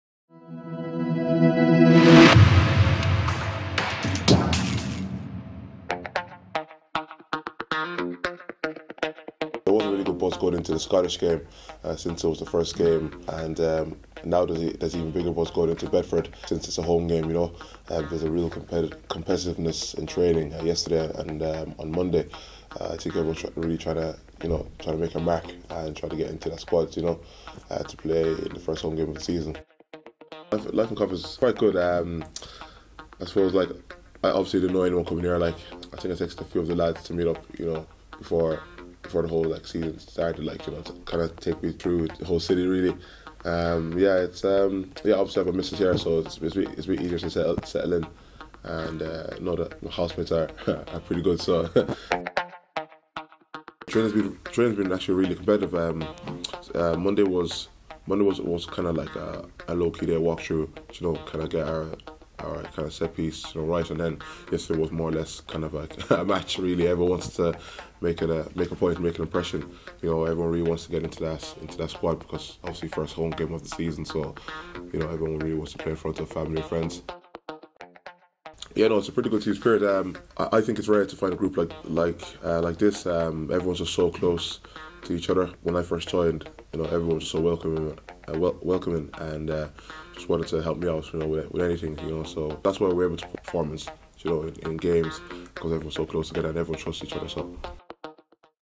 𝐖𝐄𝐄𝐊𝐋𝐘 𝐖𝐄𝐈𝐆𝐇-𝐈𝐍 with WOMAG | Daniel Okeke
𝐖𝐄𝐄𝐊𝐋𝐘 𝐖𝐄𝐈𝐆𝐇-𝐈𝐍! ⚖️

Daniel Okeke features in this week’s Weekly Weigh-In with Womag ⚖️ as the team prep for the first home game of the season against Bedford Blues 👊